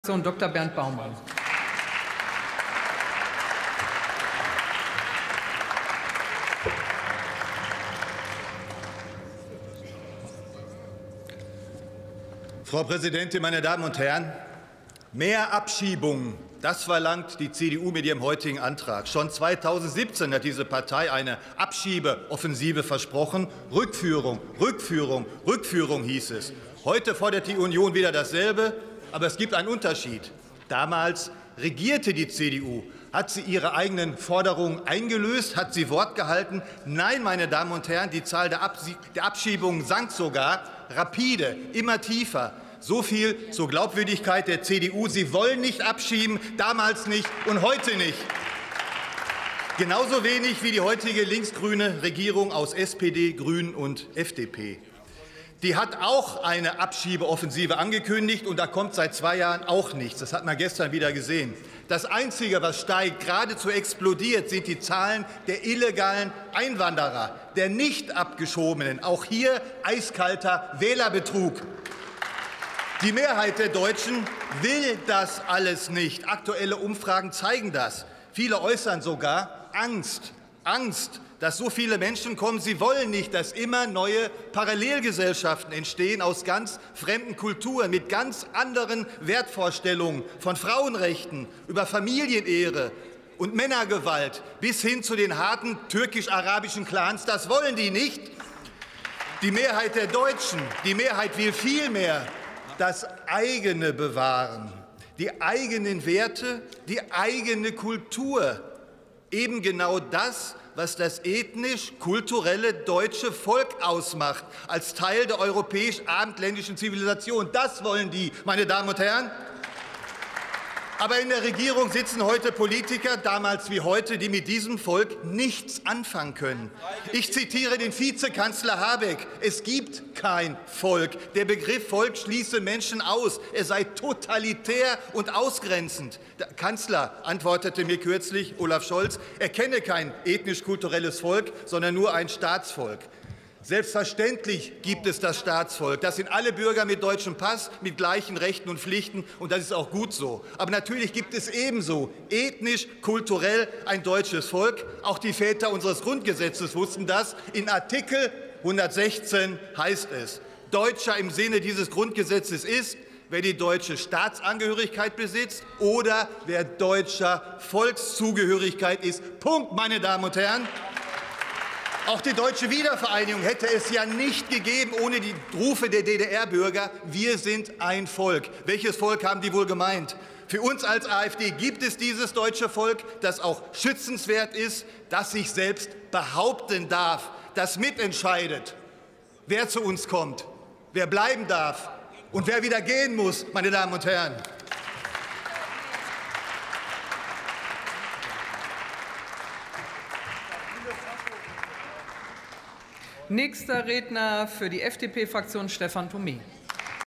🔥„Dr. Bernd Baumann "GREIFT,, 🔥CDU/CSU wegen Nichterfüllung ihrer eigenen Abschiebeforderungen an"✈️
Abgelehnte Anträge der AfD 🔥„Dr. Bernd Baumann "GREIFT,, 🔥CDU/CSU wegen Nichterfüllung ihrer eigenen Abschiebeforderungen an"✈️

🔥📣 Auch zwei Anträge der AfD-Fraktion für eine „nationale Kraftanstrengung zur Abschiebung von abgelehnten Asylbewerbern" (20/1508) und für eine „Rückführungsoffensive 2023" (20/6184) fanden keine Mehrheit im Parlament.

✈️✅️Darin forderte die AfD-Fraktion die Bundesregierung auf, die Ausreise abgelehnter Asylbewerbern „mit allen zur Verfügung stehenden Mitteln zu forcieren und über geeignete Anreizsysteme verstärkt zu fördern" sowie durch wirksamere Grenzkontrollen an europäischen und deutschen Außengrenzen die Wiedereinreise solcher Asylbewerbern zu unterbinden.

🔥📣Auch sollte die Bundesregierung nach dem Willen der AfD gegenüber Herkunftsstaaten ausreisepflichtiger Ausländer, die bei der Rücknahme ihrer Staatsbürger nicht oder nur unzureichend kooperieren, Maßnahmen im Bereich der Visavergabe, der Handelspolitik, des Technologietransfers und der Entwicklungszusammenarbeit „konzertiert" einsetzen. Der Bundestag wies die beiden Vorlage auf Grundlage von Beschlussempfehlungen des Innenausschusses (20/6260, 20/64829) mit den Stimmen der übrigen Fraktionen zurück.

AfD: Eigene Werte und Kultur bewahren

📣🔥 Dr. Bernd Baumann (AfD) warf der CDU/CSU vor, ihrer Forderung nach Rückführungen zu ihrer Regierungszeit selbst nicht nachgekommen zu sein. Die Union wolle ebenso wie die heutige Regierungskoalition nicht abschieben, während die Zahl der illegalen Einwanderer und der nicht Abgeschobenen „geradezu explodiert". Die Mehrheit der Deutschen wolle indes „die eigenen Werte, die eigene Kultur" bewahren, die „das ethnisch-kulturelle deutsche Volk" ausmache. Rückführungen geradezu explodiert 🔥

🔥📣 In der Regierung säßen jedoch Politiker, „die mit diesem Volk nichts anfangen können". Selbstverständlich gebe es ein „Staatsvolk", zu dem alle Bürger mit deutschem Pass sowie gleichen Rechten und Pflichten gehörten. Ebenso gebe es aber auch „ethnisch-kulturell ein deutsches Volk", das schützenswert sei.

Vielen Dank, dass Sie sich dieses Video angesehen haben.